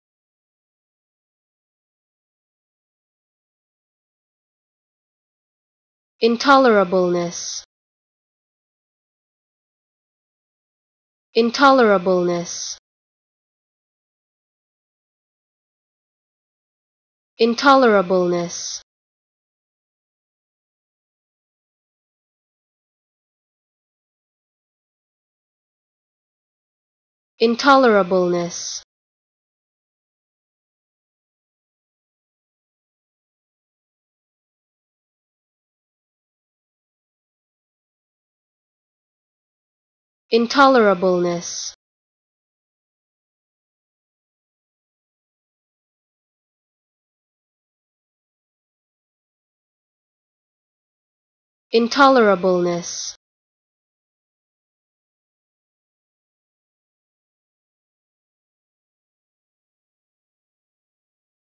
How To Say Intolerableness
Learn how to say Intolerableness with EmmaSaying free pronunciation tutorials.